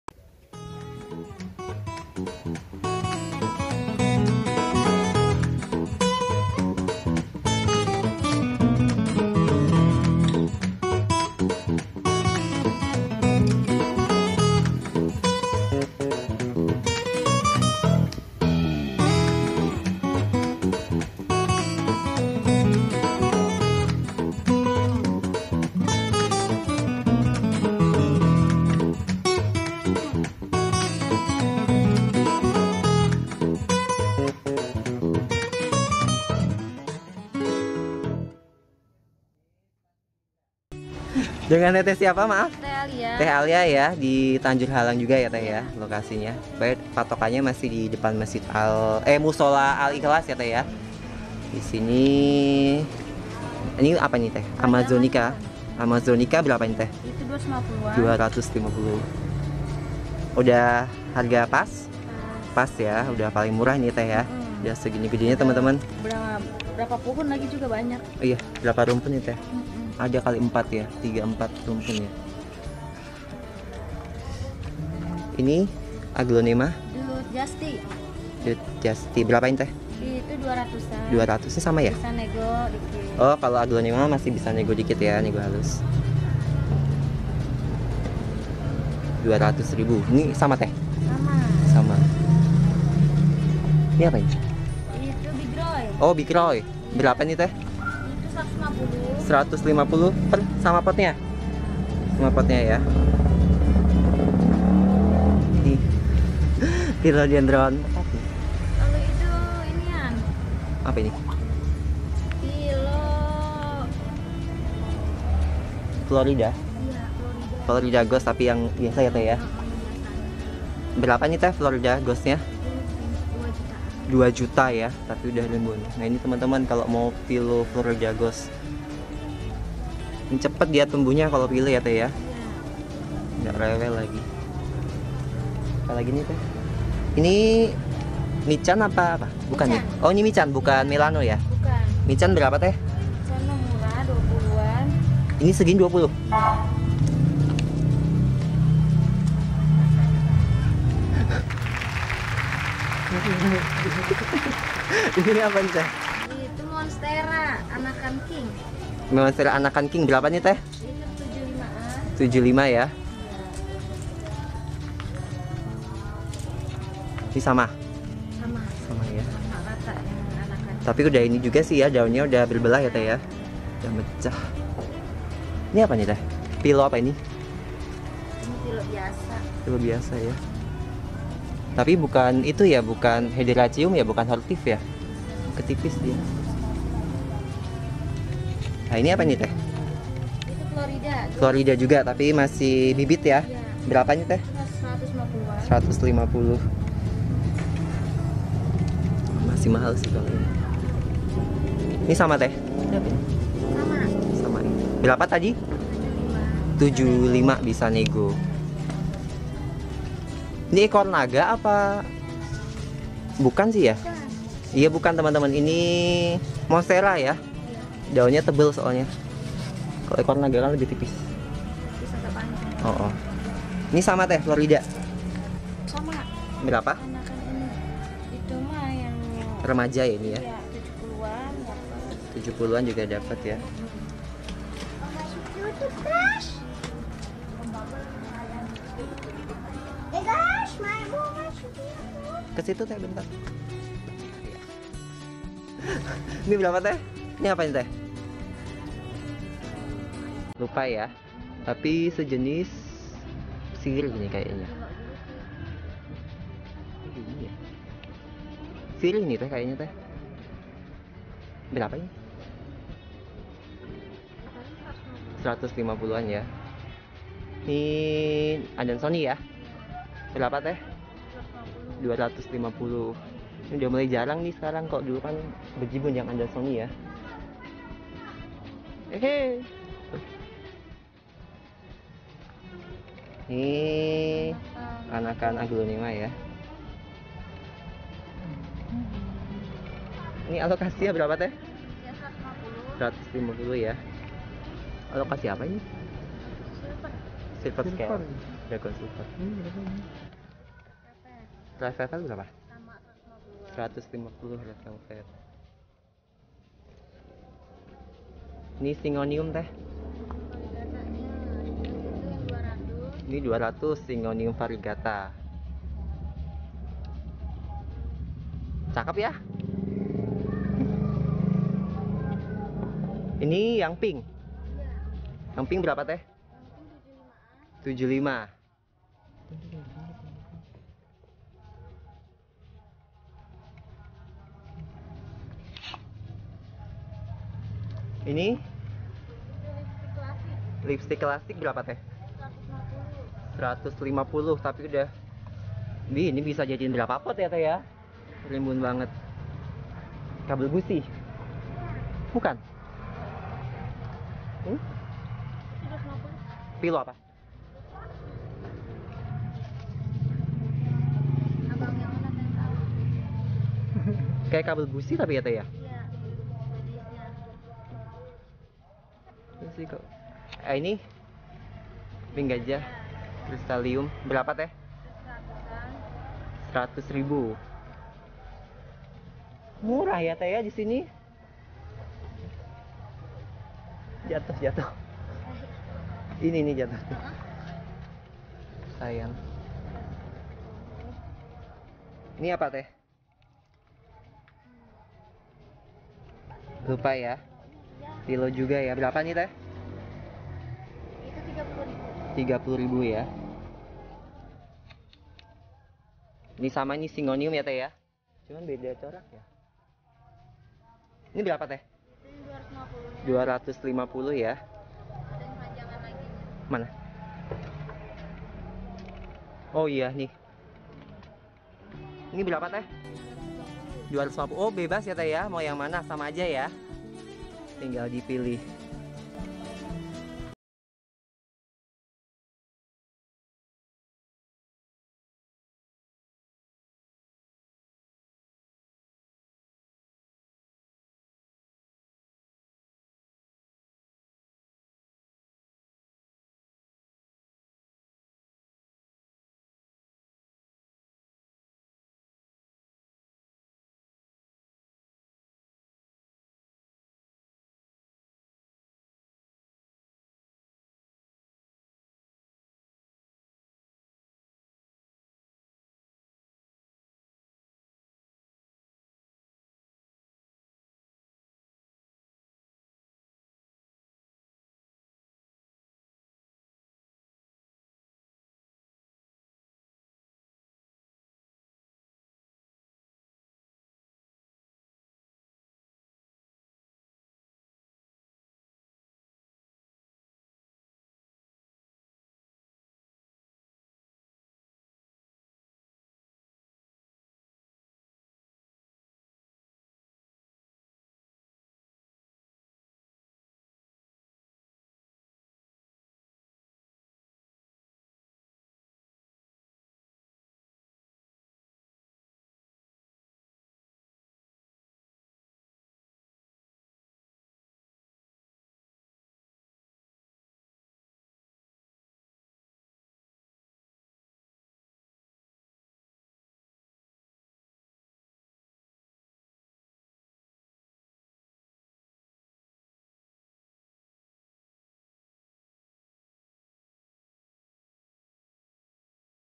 LARIS!! REVIEW HARGA TANAMAN HIAS di TAJURHALANG BOGOR MILIK TEH ALIA
Lapak tanaman hias Teh Alia:

🌱 Aqila Taman

Jalan tengah, kampung ragamukti rt 04/02.
Desa citayam, kel. tajurhalang. Bogor
(patokan depan mushola Al-Ikhlas)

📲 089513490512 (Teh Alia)